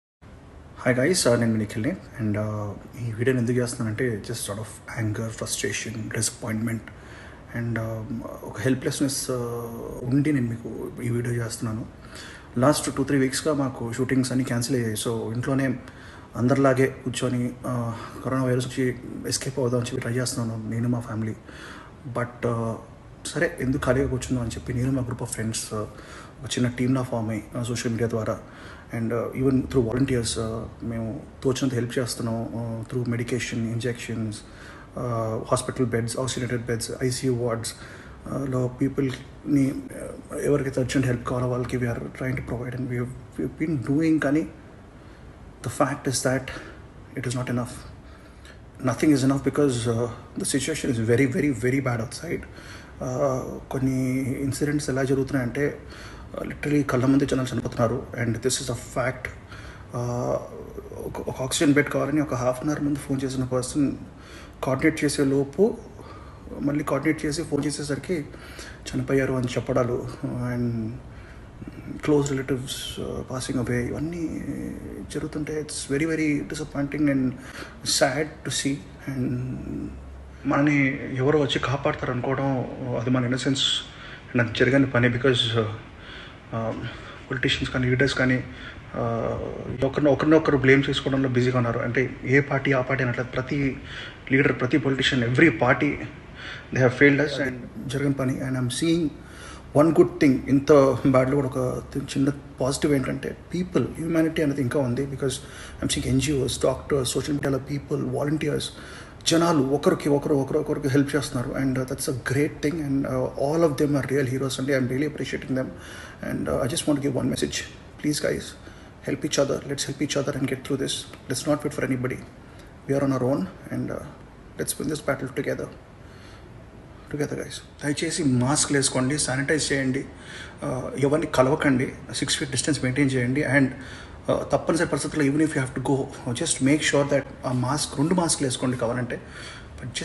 EMOTIONAL VIDEO : Hero Nikhil Becomes Emotional about India's Situation | Life Andhra Tv
Watch here  EMOTIONAL VIDEO : Hero Nikhil Becomes Emotional about India's Situation